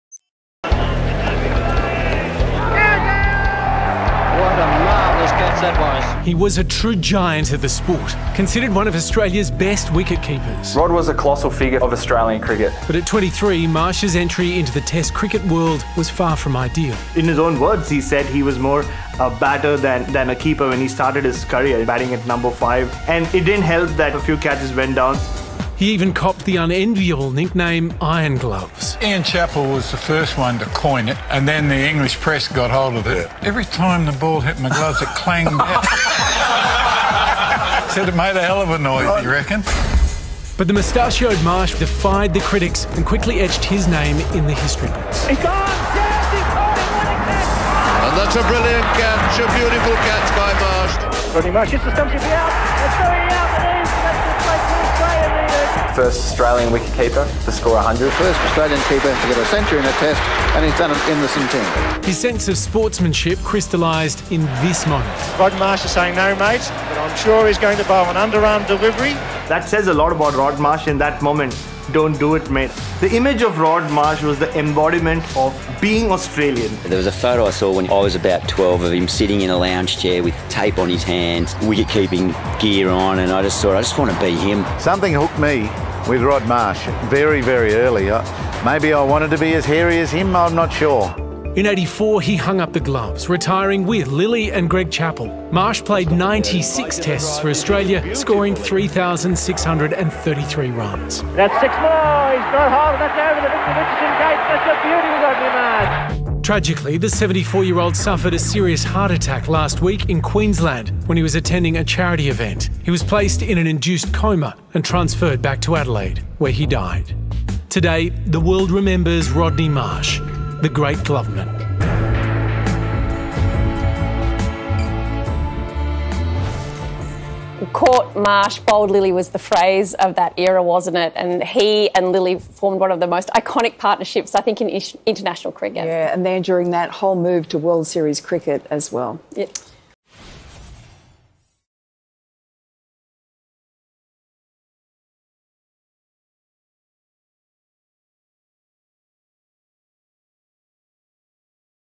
Australian Cricket Legend Rod Marsh Dies Aged 74
Today, the cricket world is in mourning as an Australian legend, Rod Marsh has died aged 74. During the 70s and 80s, Marsh personified the nation's cricket and his impact of the game was felt worldwide.